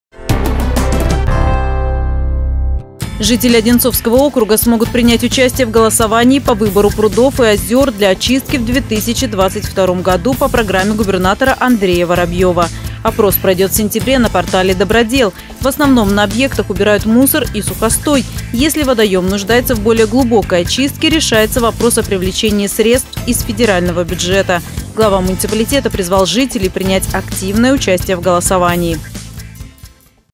Голосование по очистке прудов стартует в Подмосковье в сентябре
Жители Одинцовского округа смогут принять участие в голосовании по выбору прудов и озер для очистки в 2022 году по программе губернатора Андрея Воробьева. Опрос пройдет в сентябре на портале «Добродел».